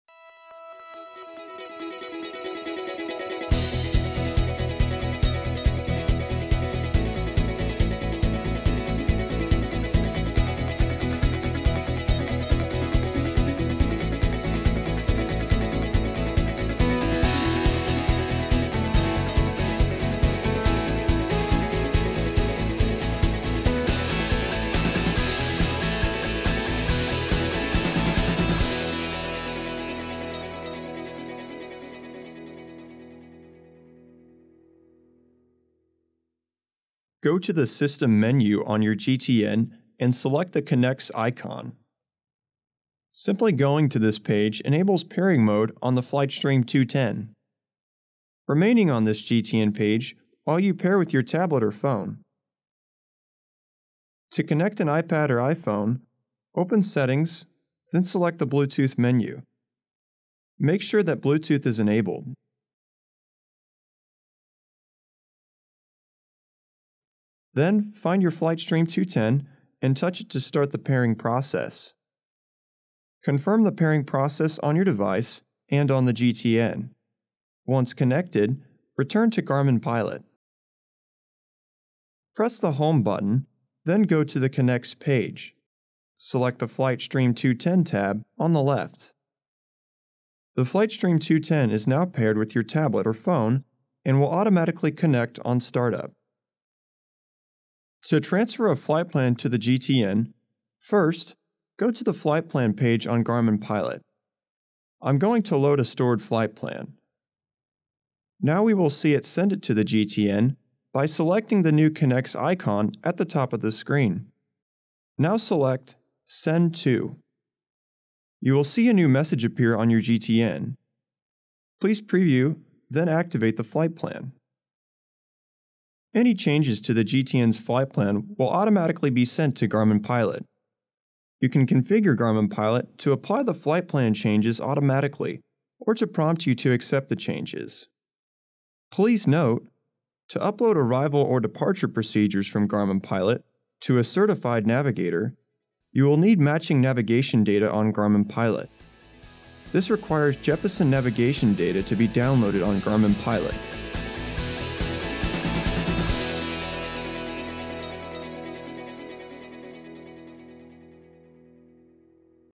Garmin Pilot™: Connect the Flight Stream 210 with Connext
Learn how to connect Garmin Pilot to your Flight Stream 210 to receive information such as traffic, weather, and GPS signal from your certified Garmin panel mounted device using the new Connext feature.